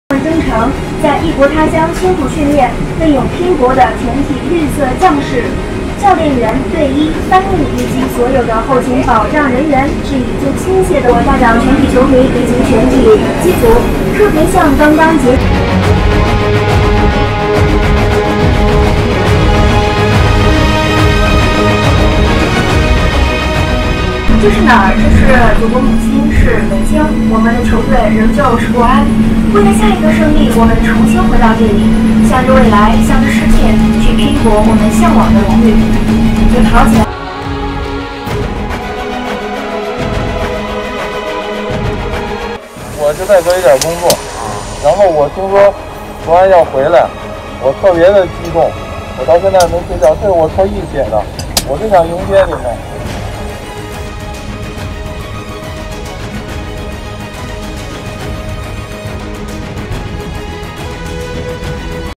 国安回家! 机场工作人员防护服写“国安是冠军” | Beijing Guoan returned home with best result of the club in ACL history
Beijing Guoan returned home on a Chartered flight from Doha on Tuesday morning after finishing the AFC Champions league competition.

Foreign players like Renato Augusto, Fernando and Jonathan Viera as well as coach Bruno Genesio were not on the flight as they will go home to start their vacation.

All the Chinese players and team staff were taken for 14-day quarantine at a hotel in Beijing once they landed the airport according to the Chinese anti-pandemic rules.

Beijing, a quarter-finalists of the Asian Champions League in Doha, achieved the best result in their history of this Continental soccer event.